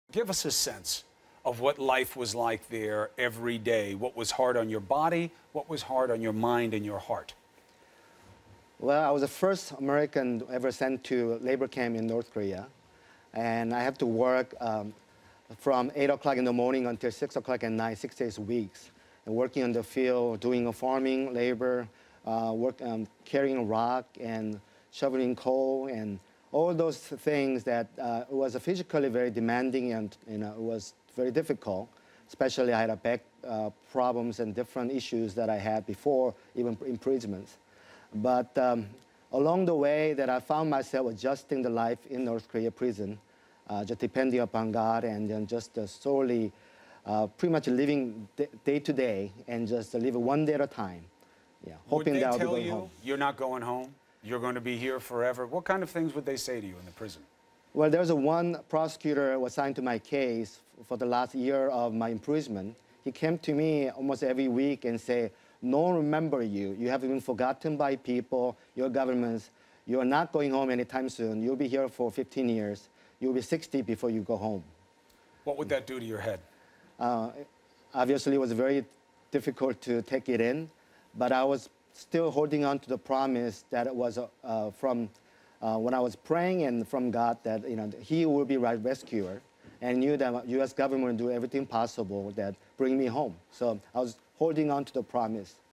Bae: They told me everyone had forgotten me
Kenneth Bae reflects on his imprisonment in North Korea in an interview with CNN's Chris Cuomo.